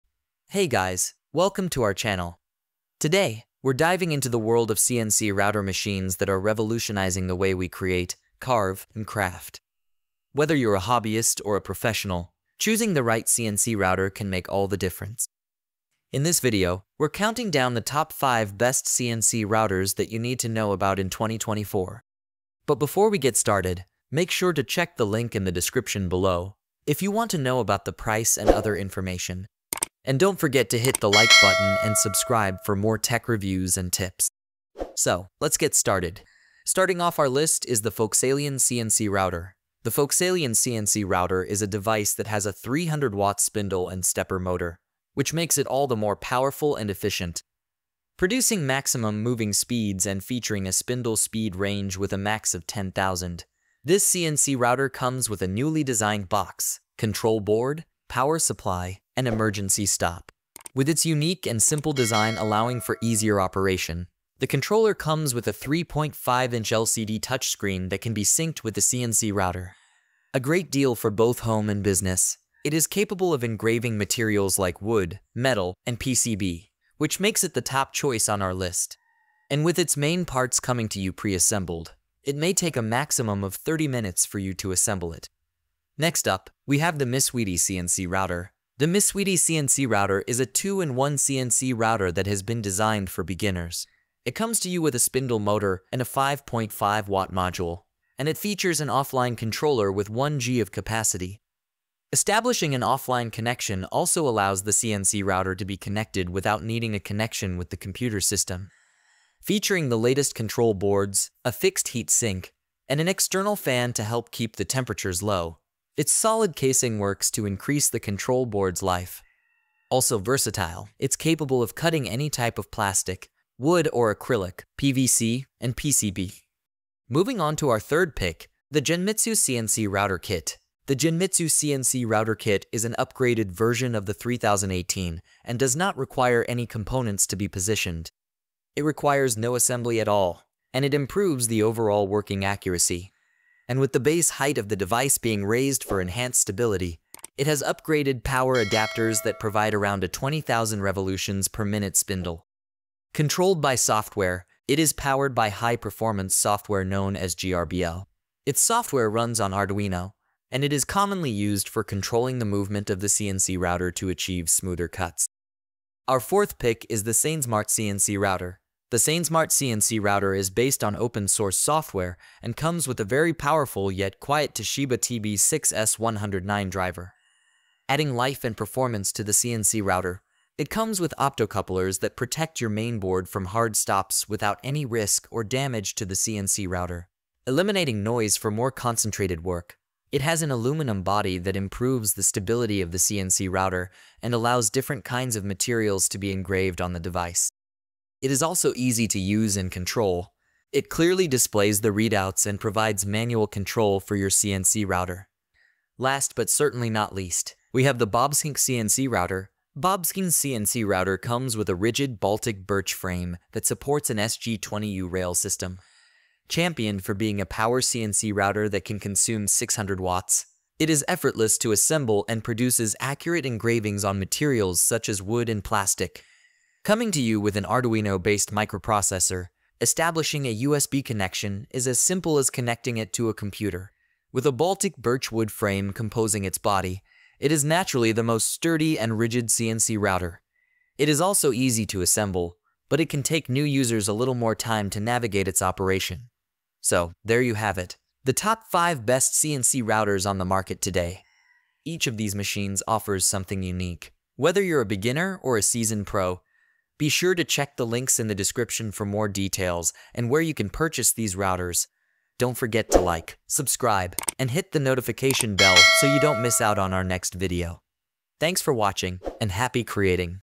Top 5 Best CNC Routers in 2024 | Best CNC Machines for Your Workshop
🔗 Check out the links to each featured Best CNC Routers below:

1️⃣FoxAlien CNC Router
2️⃣MYSWEETY CNC Router
3️⃣Genmitsu CNC Router Kit
4️⃣SainSmart CNC Router
5️⃣BobsCNC CNC Router

Looking for the best CNC router to take your woodworking, metalworking, or crafting projects to the next level? In this video, we review the top 5 CNC routers of 2024 that offer precision, durability, and versatility. Whether you're a hobbyist or a professional, these CNC machines are perfect for your workshop.

Best Buy USA is a participant in the Amazon Services LLC Associates Program. I earn through amazon associates from qualifying purchases.